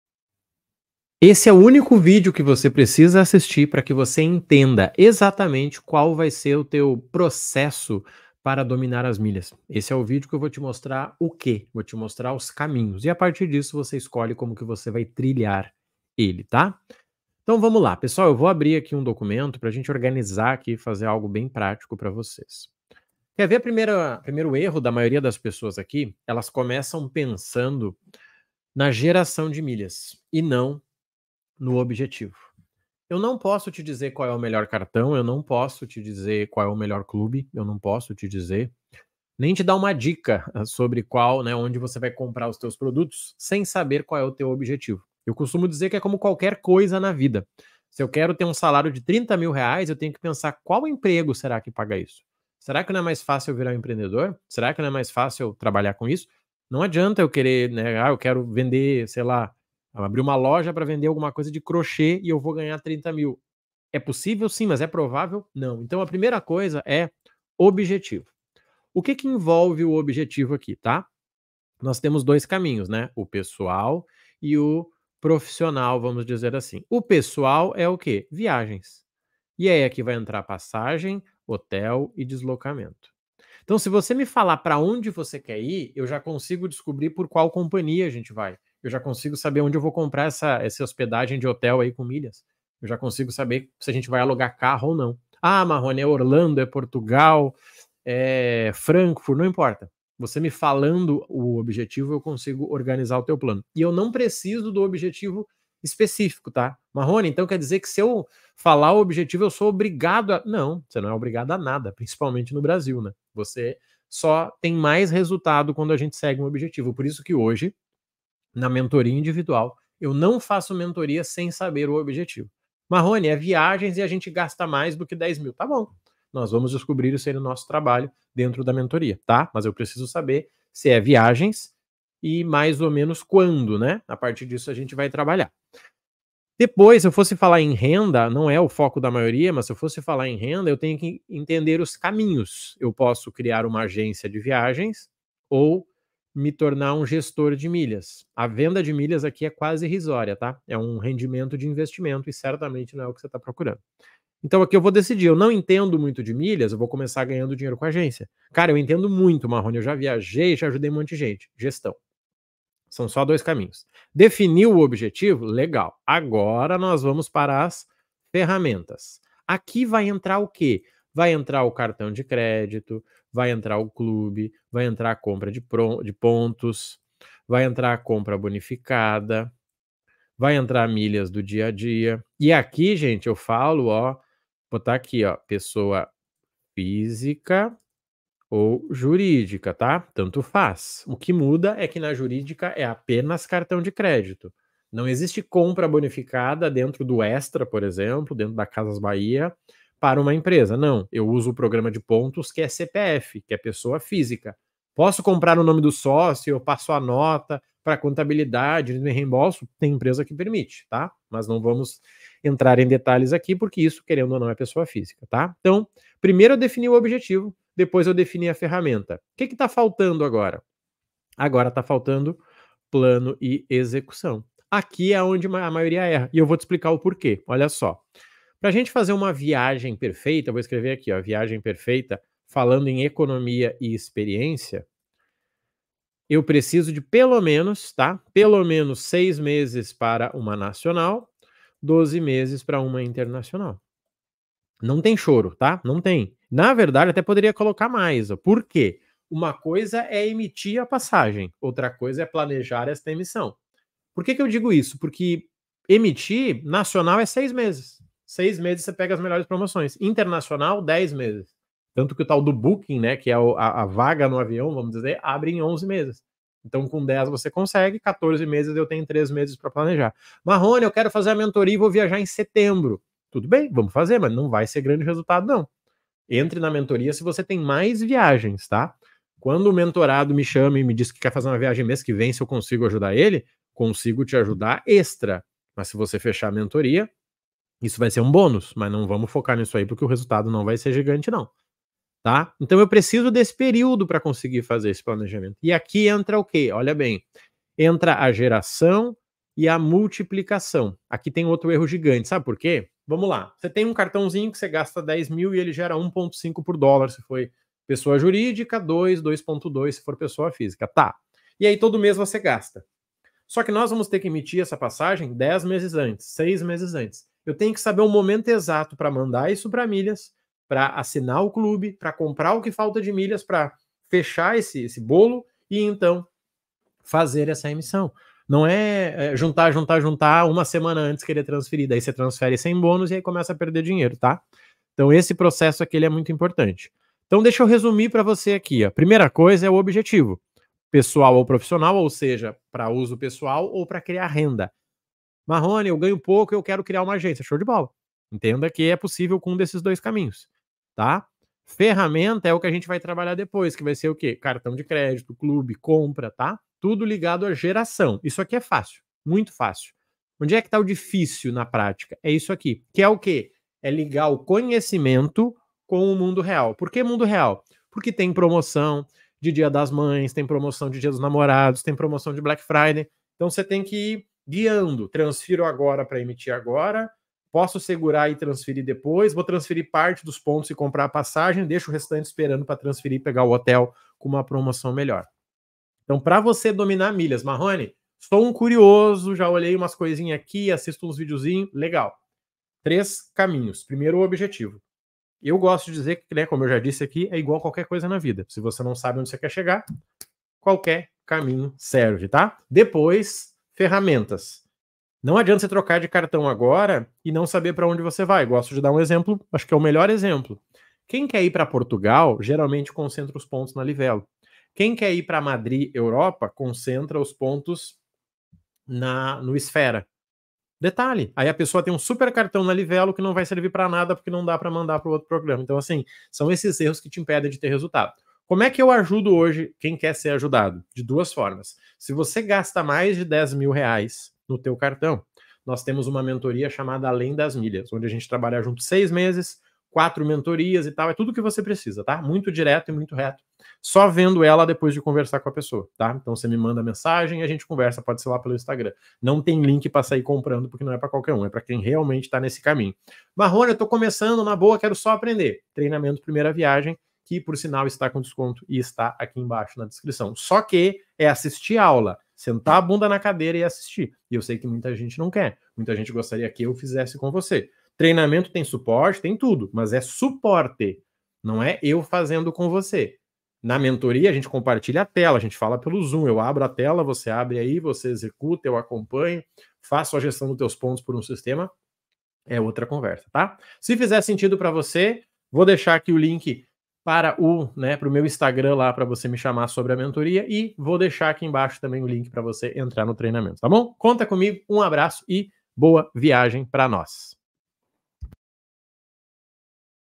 Como dominar 100% das MILHAS para ter resultados para VIAGENS ou RENDA? Entenda os 3 pessos
Quer ter acesso há um BALCÃO DE MILHAS para comprar e vender milhas?
Receber em primeira mão a análise de promoções e alertas de viagens baratas?
Faça parte da Comunidade Milhas 3x